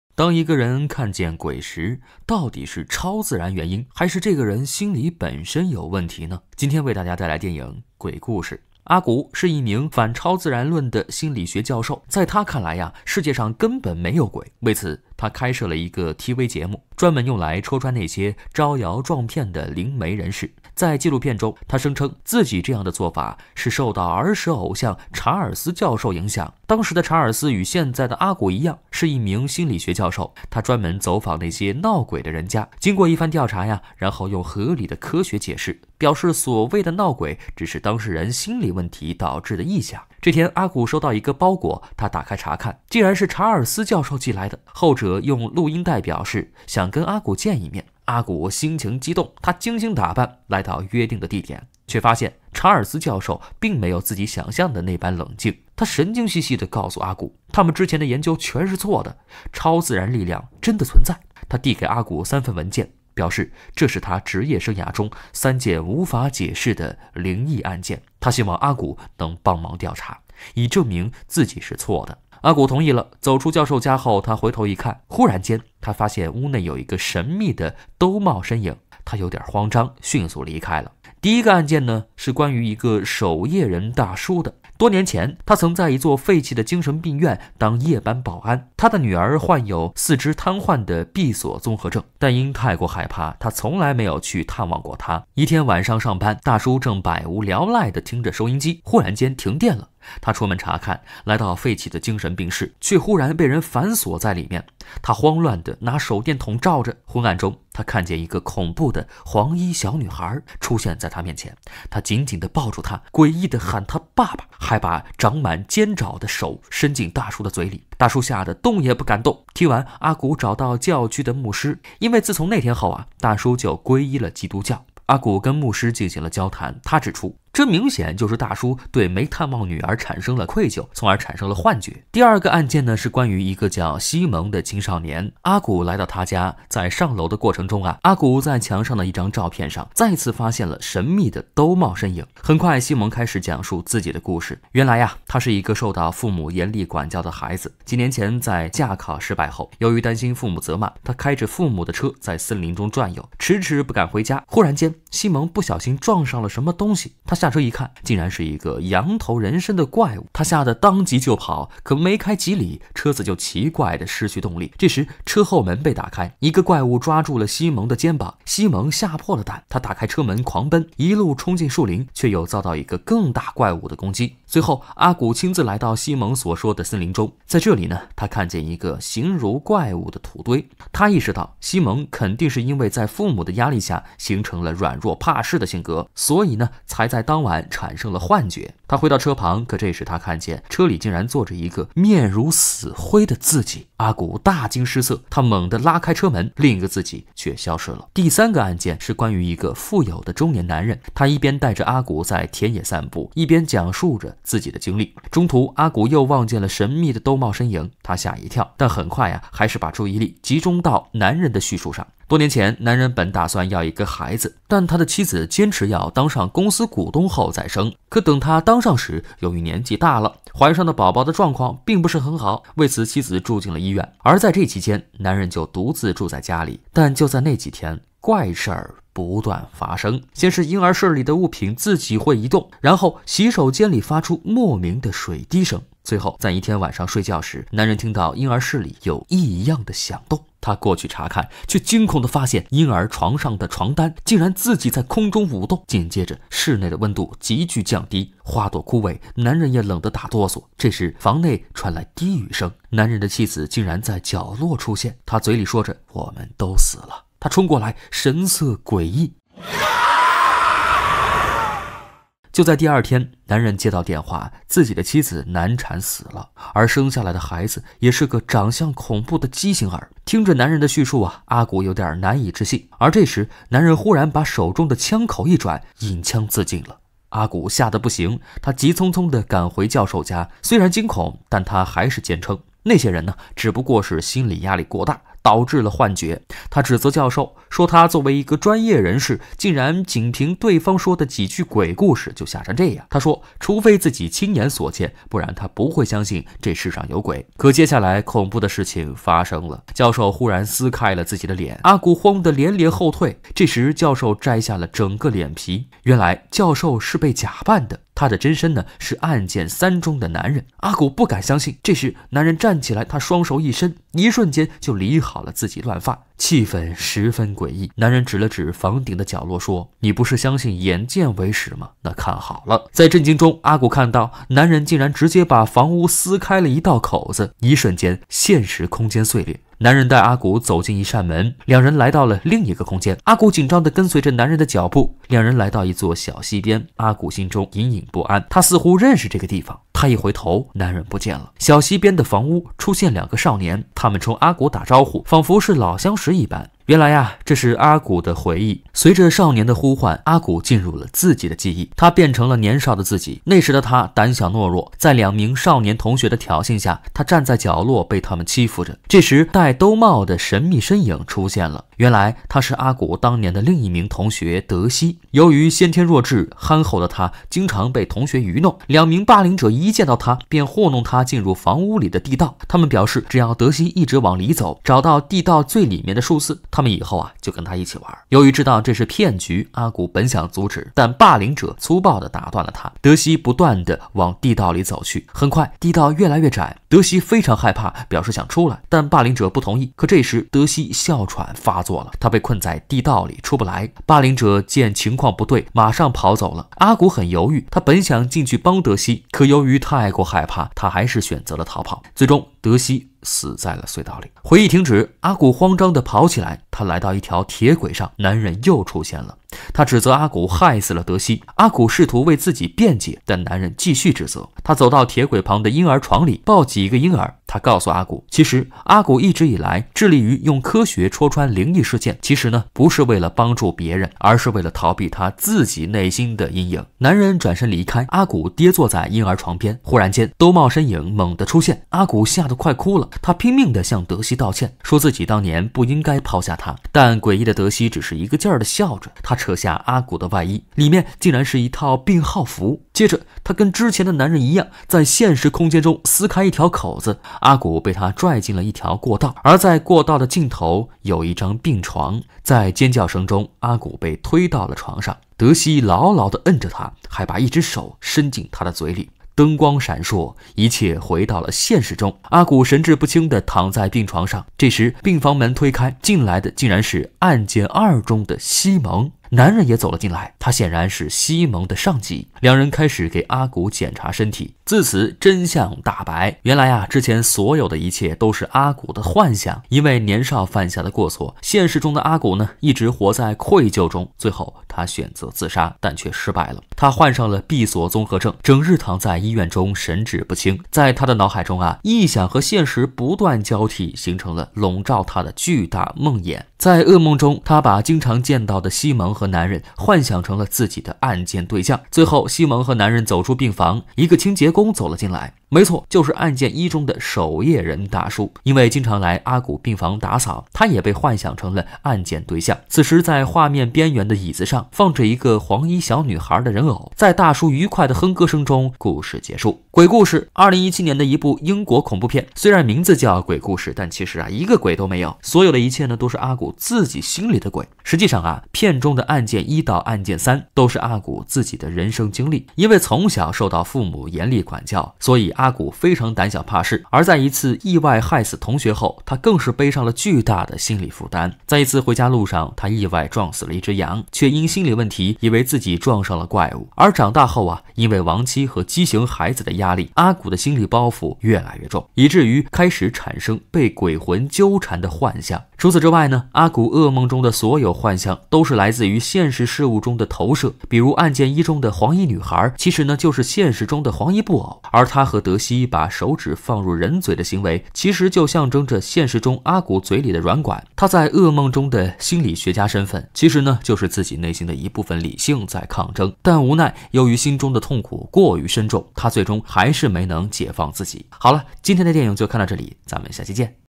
男人因儿时的过错，终生活在阴影里，最终选择走上绝路
男人因小时候的一个过错无法自拔，终生活在阴影里